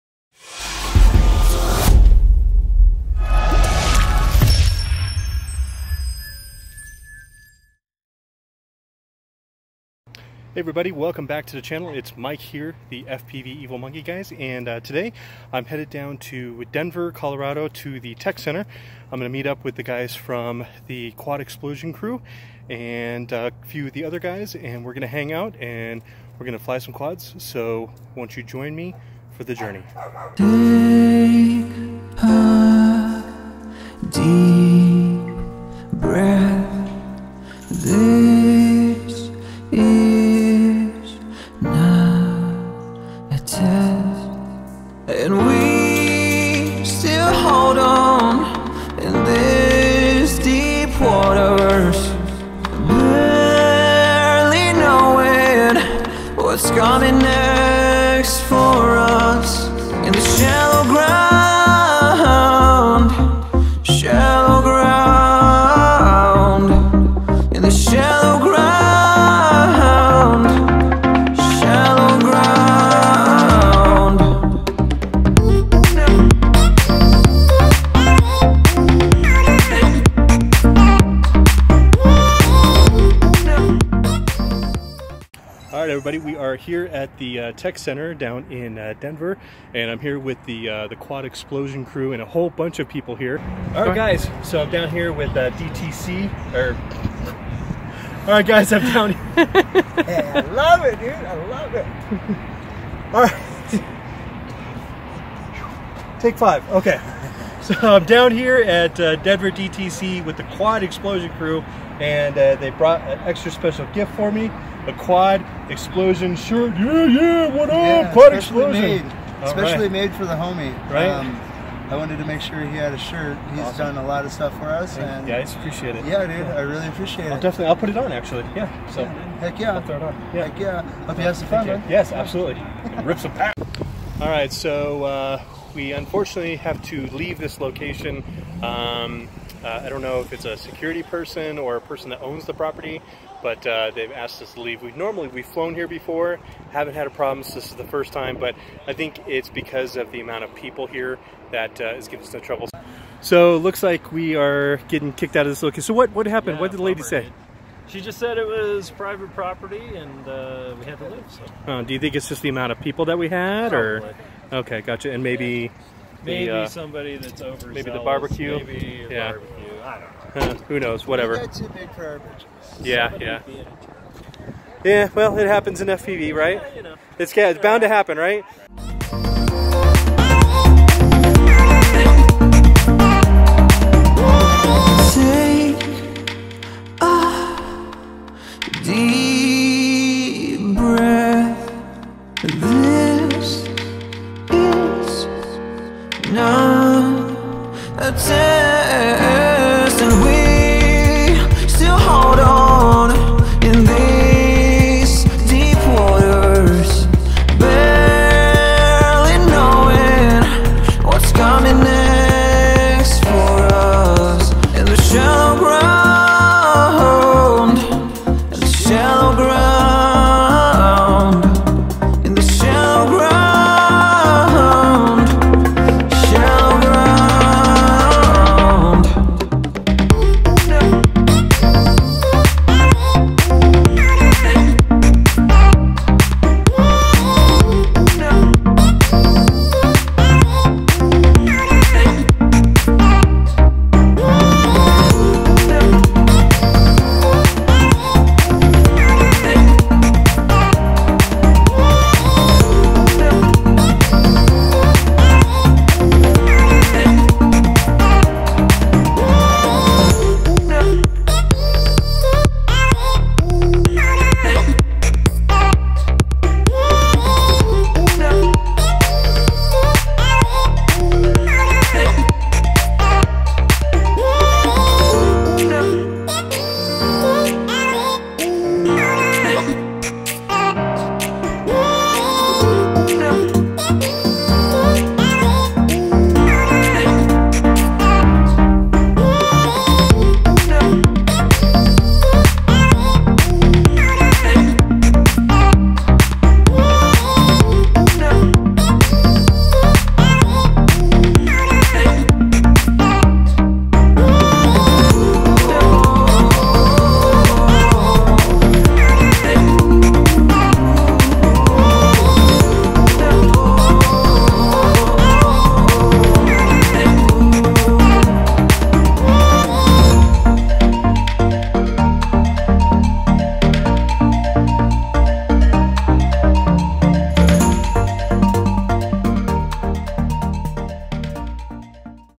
We Got Kicked Off The Property
We got kicked off the property we came to fly. It happens in FPV. Oh well at least the day was not a complete loss. I was able to find a couple of spots to rip a few packs on the way home. Also a huge thanks to Quad Xplosion for the shirt. And my viewer SpaceCowboy FPV for coming up to visit.

My Freestyle Rig
iFlight XL5
Matek F405 CTR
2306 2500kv
GemFan 5043 Wind Dancers
Foxeer Predator
AKK Ultimate Vtx

Amazon Influencer Shop